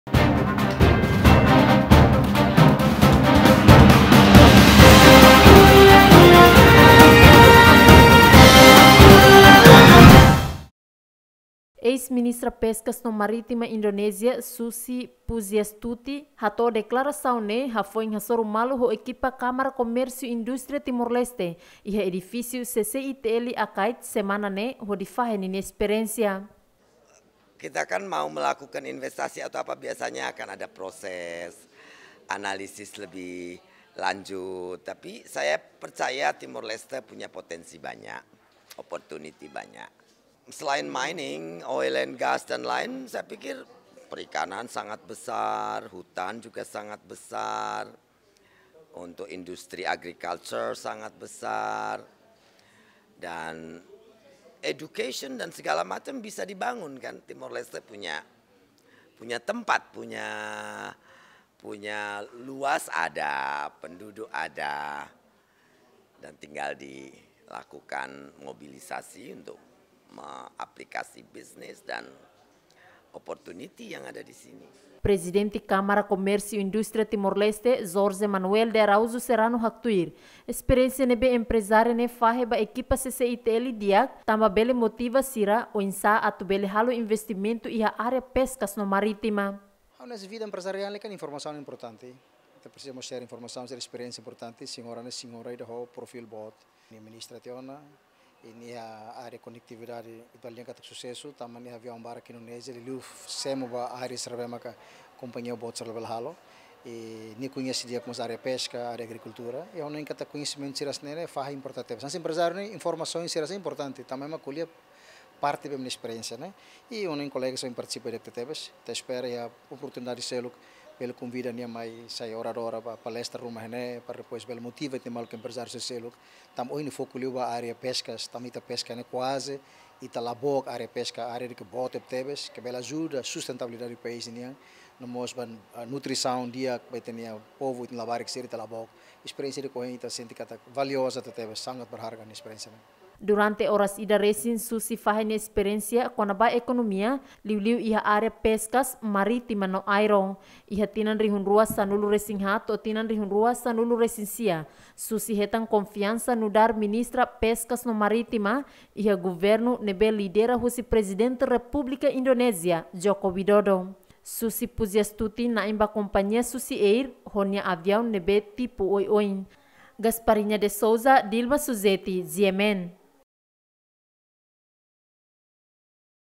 Susi PujiAstuti: TL iha área potensiál no oportunidade barak iha área Ekonomia
Susi PujiAstuti konsidera TL iha área potensiál no oportunidade barak iha área Ekonomia